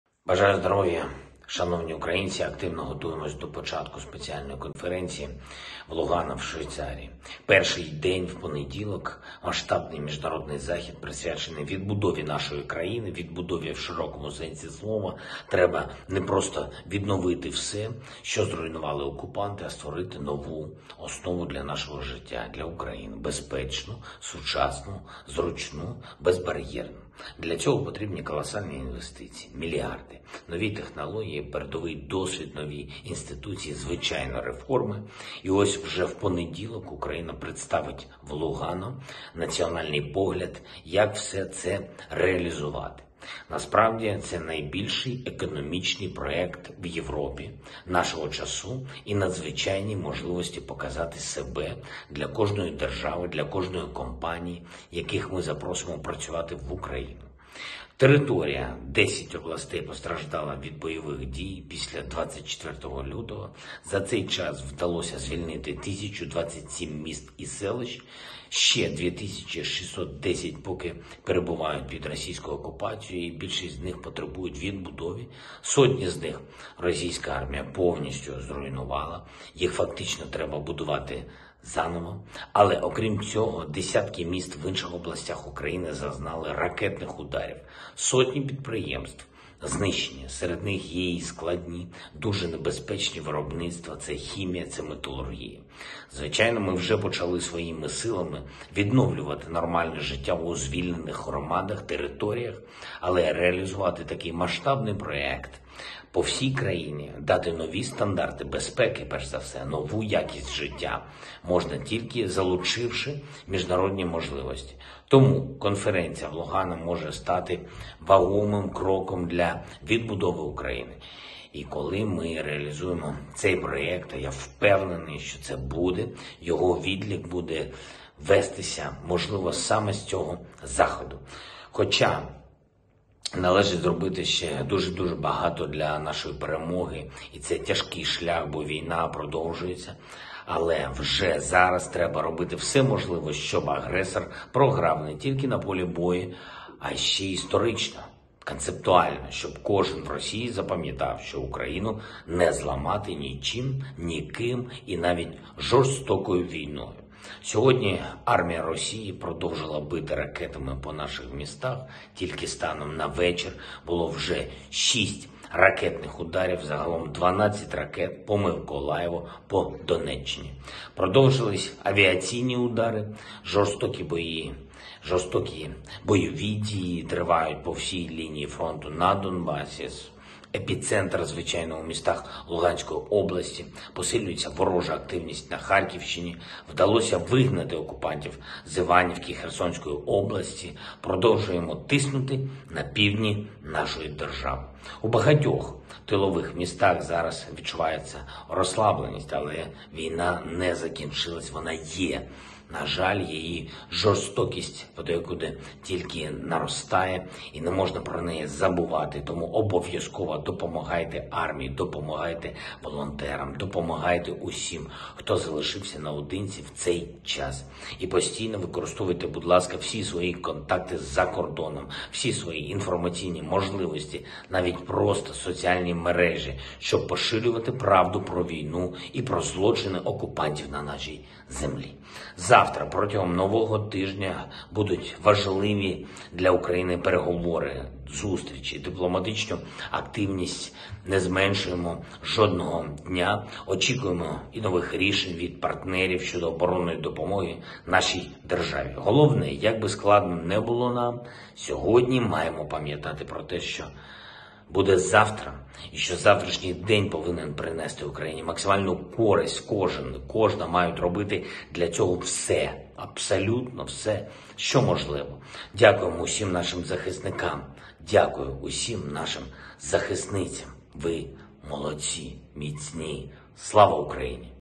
Володимир Зеленський звернувся до українців наприкінці 129-го дня війни
«У багатьох тилових містах зараз відчувається розслабленість, але війна не закінчилась, її жорстокість подекуди лише наростає, і не можна про неї забувати. Тому обов'язково допомагайте армії, допомагайте волонтерам, допомагайте всім, хто залишився наодинці в цей час. І постійно використовуйте всі свої контакти за кордоном, усі свої інформаційні можливості, навіть просто соціальні мережі, щоб поширювати правду про війну та про злочини окупантів».